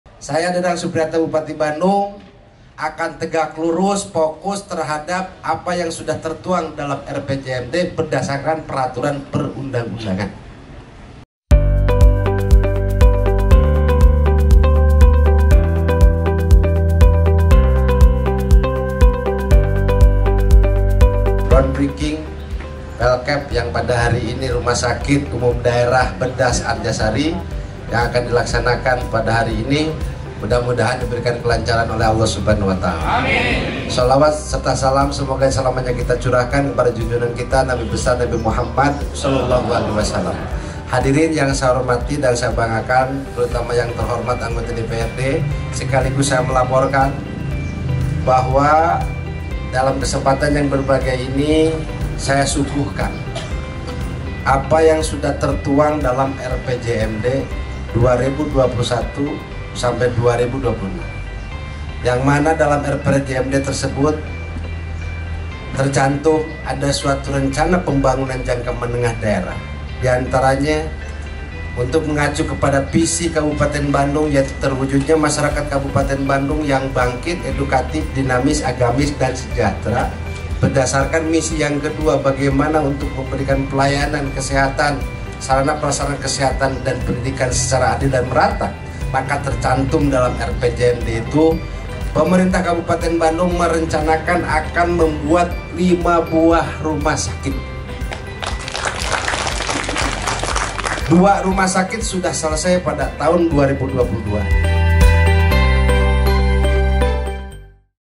Ground Breaking Ceremony Pembangunan RSD Banjaran
Bupati @dadangsupriatna menyebutkan, dengan spirit BEDAS serta upaya dan kerja keras bersama, ke depan pembangunan di bidang kesehatan akan semakin merata di semua wilayah Kabupaten Bandung. Hal ini akan mengurangi kesenjangan antar wilayah secara utuh dan terealisasi sebagaimana yang diharapkan.

"Pembangunan RSUD BEDAS Arjasari ini merupakan upaya nyata Pemkab Bandung dalam meningkatkan derajat kesehatan dan angka harapan hidup bagi masyarakat. Dengan hadirnya RSUD baru ini diharapkan pula menjadi salah satu simbol kemajuan pada dunia kesehatan di Kabupaten Bandung," ucapnya.